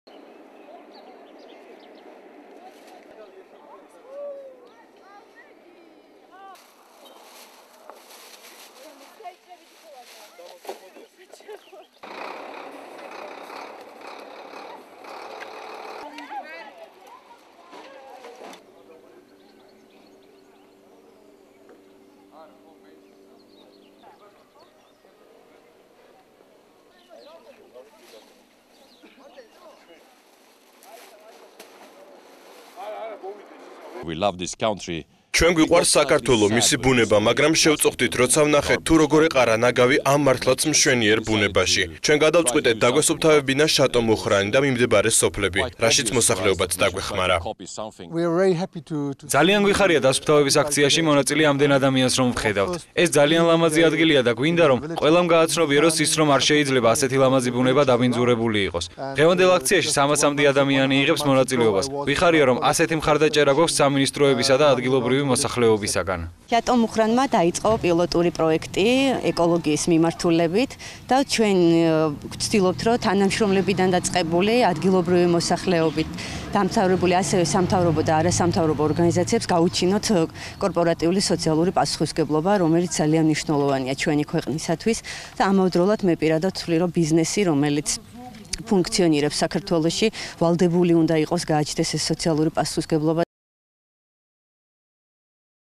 Clean up day at Chateau Mukhrani - TV3
On the 16th of May company "Chateau Mukhrani" organized clean up day in Mukhrani together with: Ministry of Environment and Natural Resources Protection of Georgia, employees of local municipalities of Mukhrani and nearby villages, Green Movement, teachers and pupils, National Wine Agency, Georgian National Tourism Administration, TBC Bank, NGO's working on environmental issues.

The day has been finished with premium wines of Chateau Mukhrani and traditional Georgian Supra